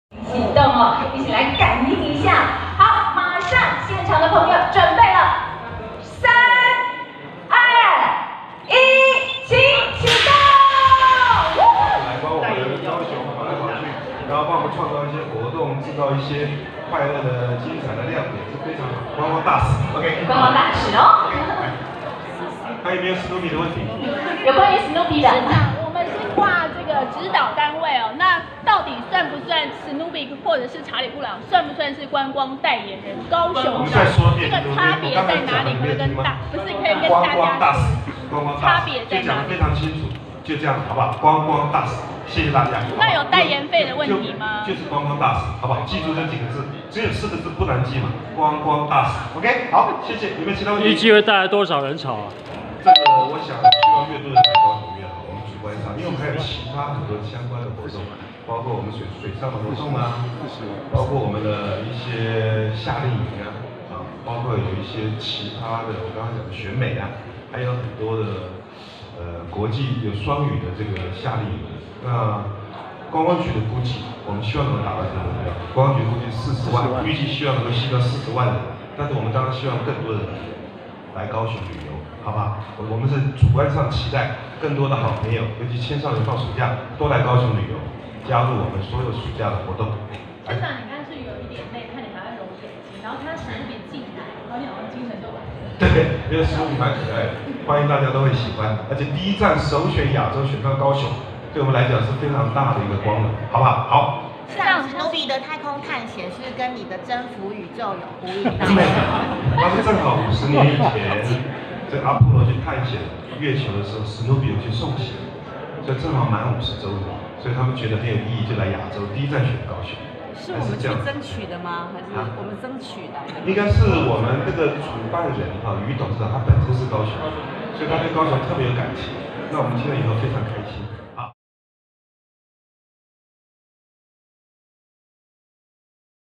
史努比是高雄觀光代言人？ 韓國瑜更正：是「觀光大使」
Snoopy（史努比）高雄城市探險活動將於暑假在高雄登場，外傳史努比將擔任高雄暑假觀光代言人？韓國瑜今天下午參加宣傳記者會，特別澄清史努比是「觀光大使」。